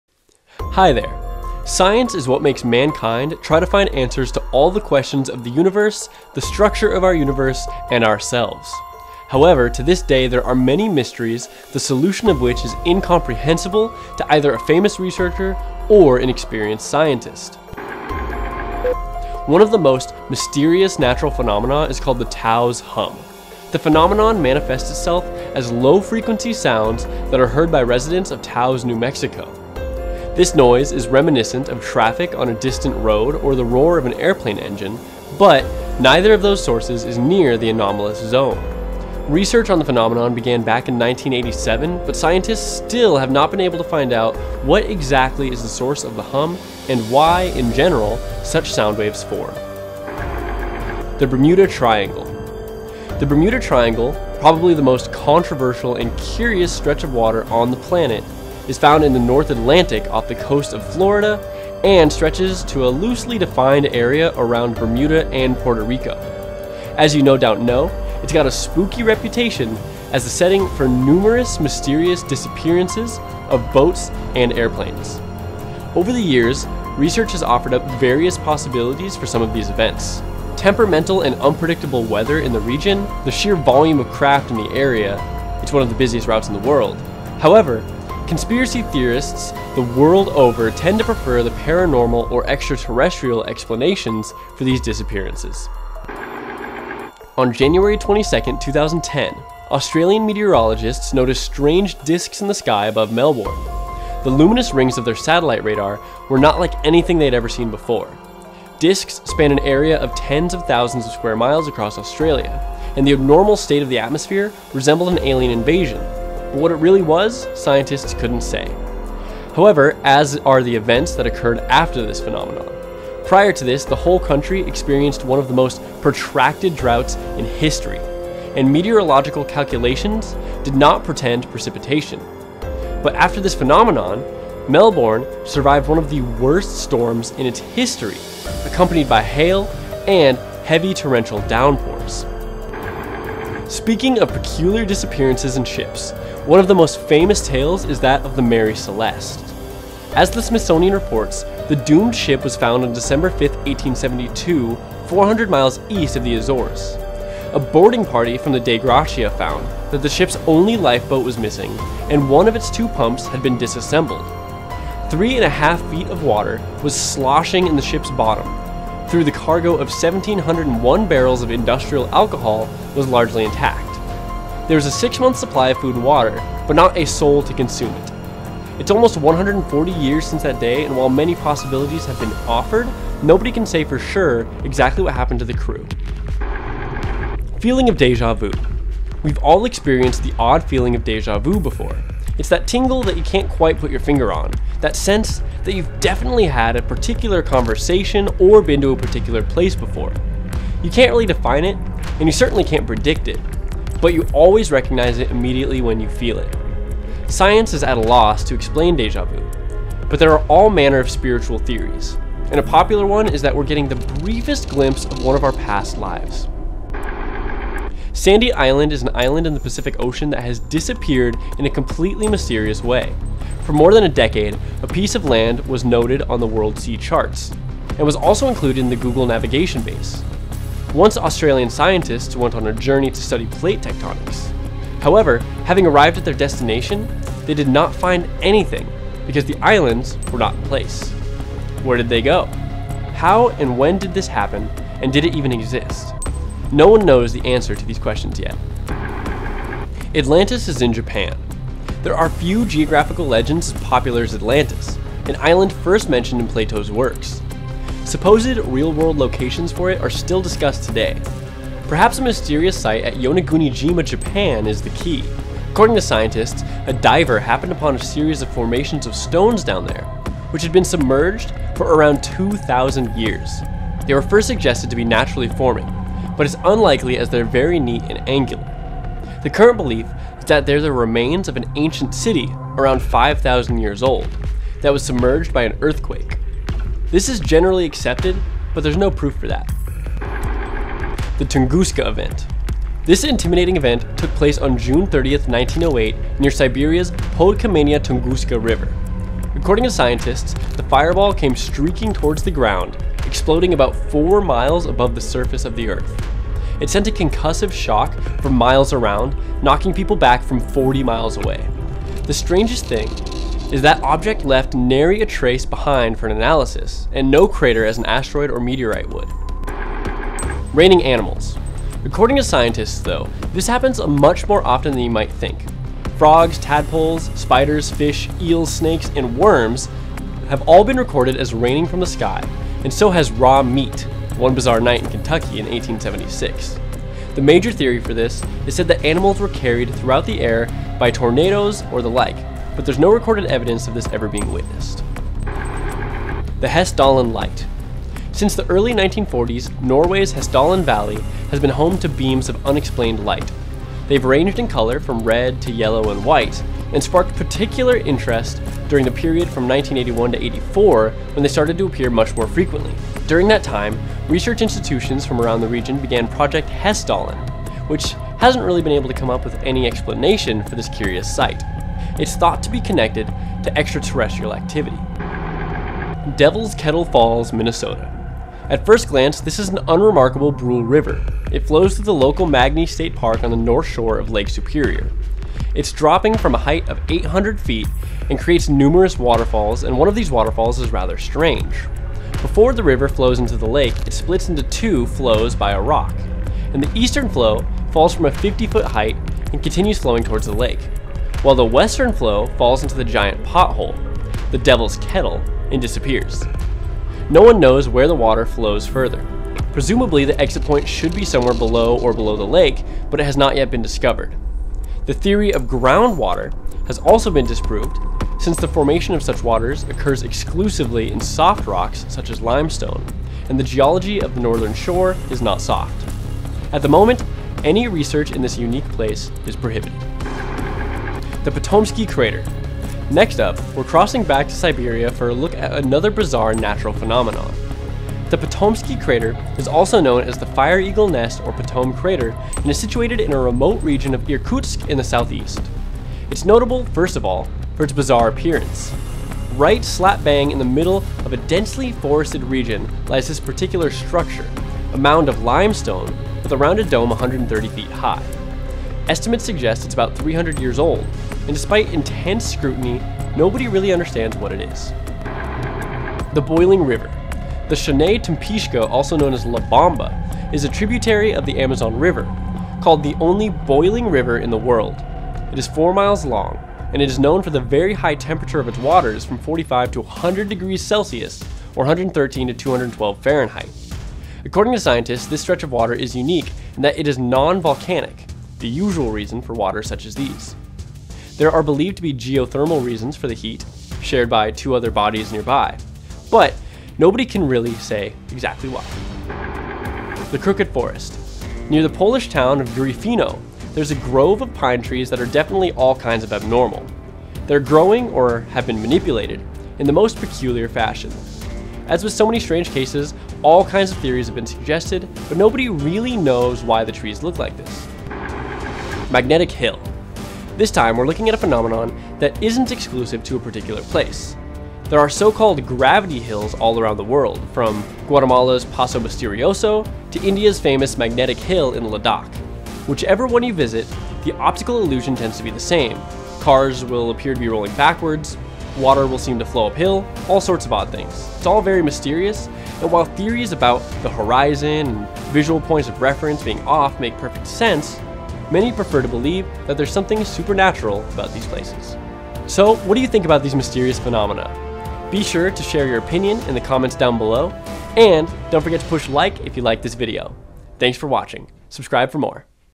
15 Most Mysterious Things That Scientists Still Can't Explain
Science is what makes mankind try to find answers to all the questions of the universe, the structure of our universe and ourselves. However, to this day there are many mysteries, the solution of which is incomprehensible to either a famous researcher or an experienced scientist.